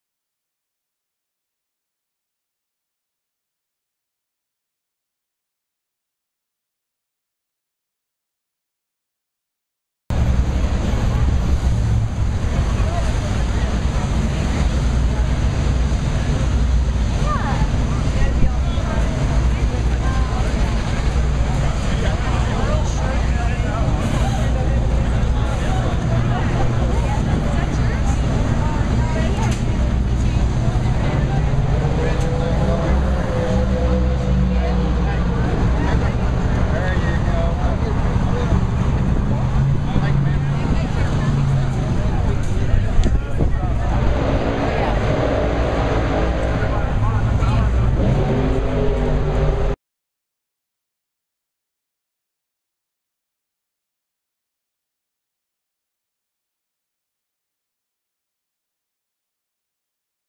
Alexis DeJoria Tequila Patrón Dallas TweetUp
Great crowd of fans and Twitter followers attended the Saturday morning TweetUp in the Tequila Patrón Funny Car pits at Texas Motorplex in Dallas. The Tequila Patrón Funny Car is driven by Alexis DeJoria. (@AlexisDeJoria).